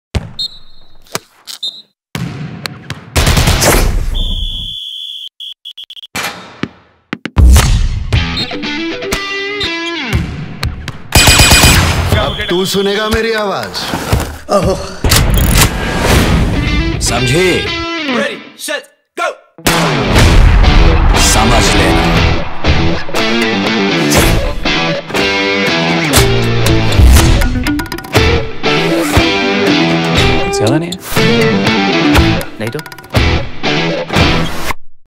MAY on Star Gold
Promo: MAY Monthly
Duration: 35 seconds
Promo Producer/Editor: Nishant Samant
Channel: Star Gold (UK)